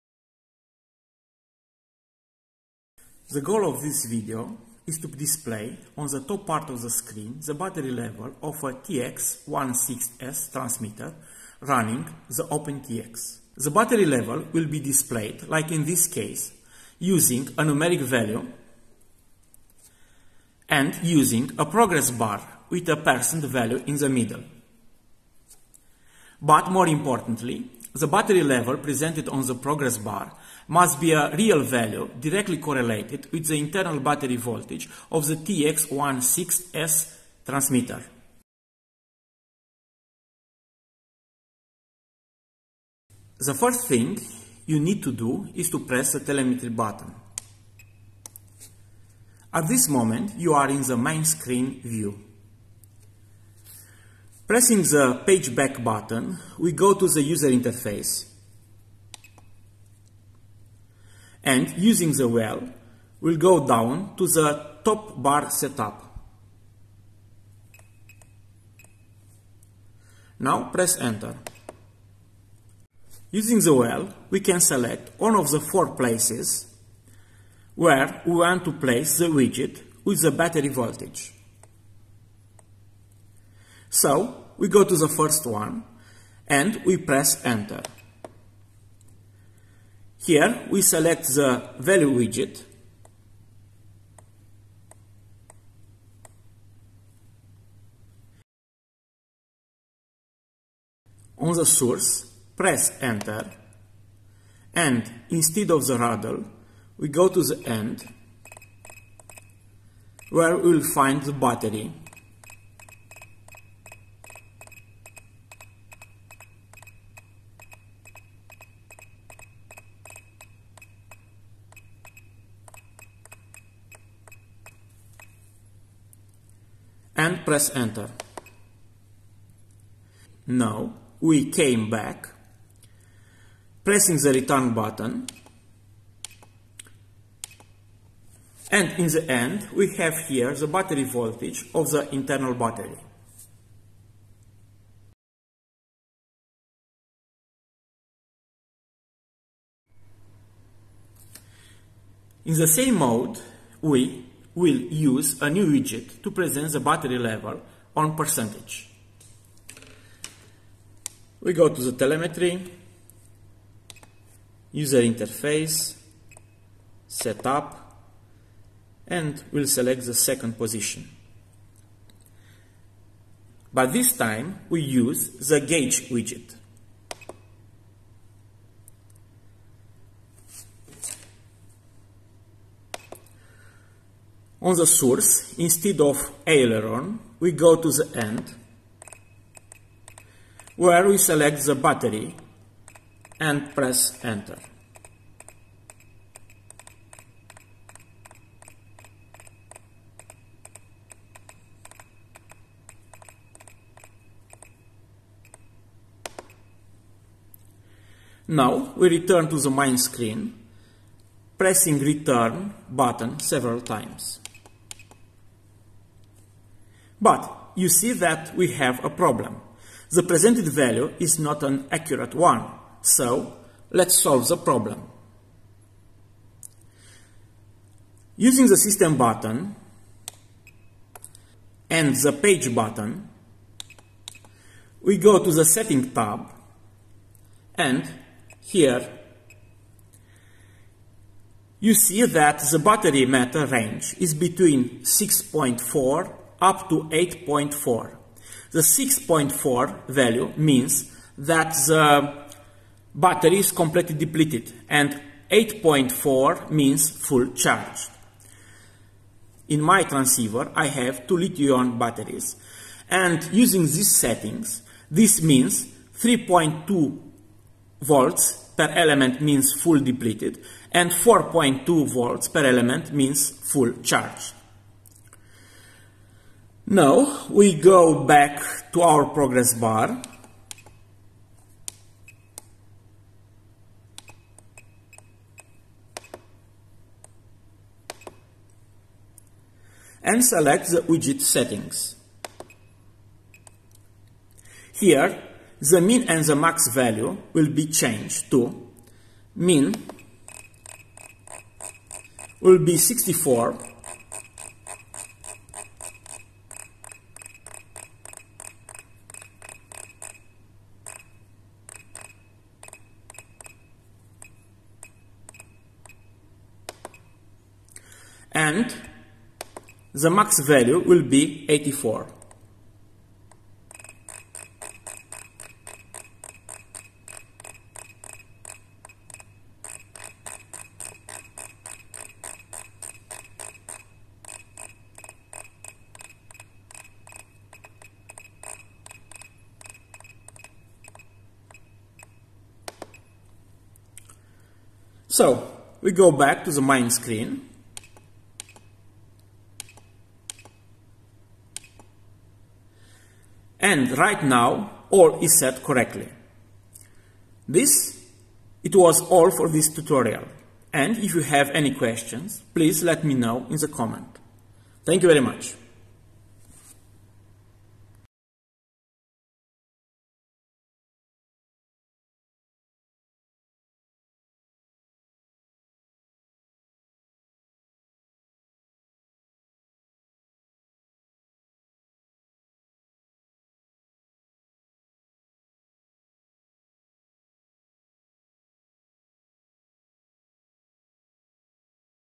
TX16S: displaying the battery level on OpenTX & EdgeTX – percentage [%] and value [voltage]
This video is a short tutorial that present two ways to display the battery level on a TX16S transmitter running OpenTX.

00:00 – Short Introduction
00:38 – Displaying the battery level as a value
02:26 – Displaying the battery level as a progress bar

1. In this video, I have Li-Ion. If you search the net, they consider that 3.0V must be the cutoff at room temperature. But, you go to the outside where can be wind and low temperature - the winter is coming 🙂. So, I decided that to take a more conservative value of 3.2V. I have 2 elements, so I choose 6.4V.
2. If you use a Li-Po battery, the voltage must not be below 3.5V per cell. Even so, many recommend not being below 3.6V. In conclusion, if you use Li-Po use 72 as a minimum value.

╠╗║╚╝║║╠╗║╚╣║║║║║═╣    I Really Appreciate Your Support!